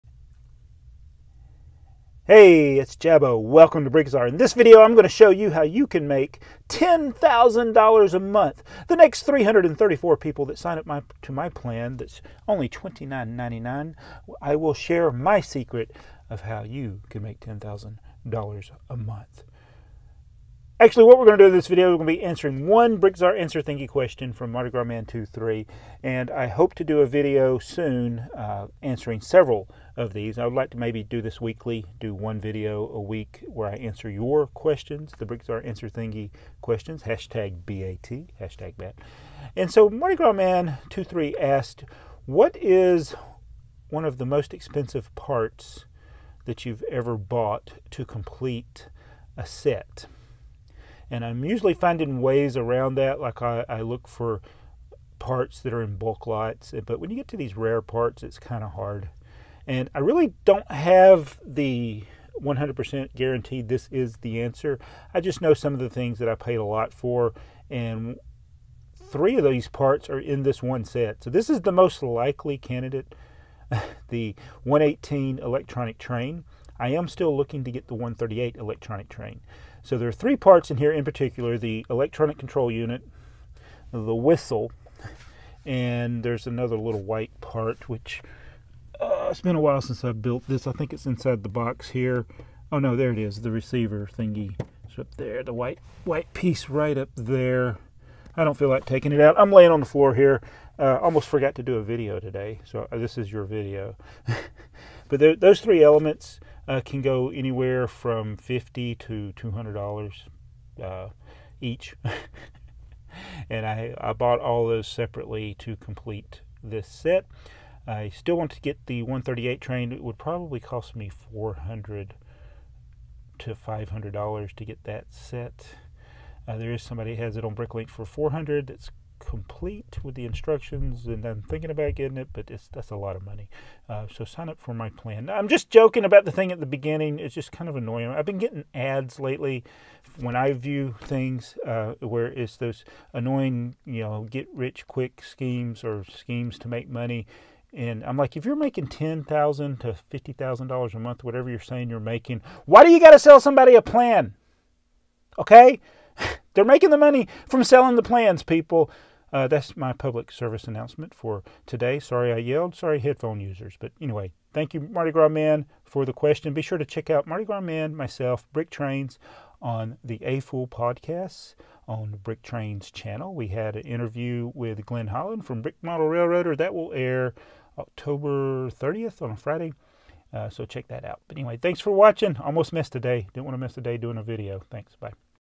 Expensive Rare LEGO Parts
Question from Mardigrasman23. Most I paid for parts to complete a set
📭 Mail to:
BrickTsar
66 Fire Tower Rd NW #264

➡️My eBay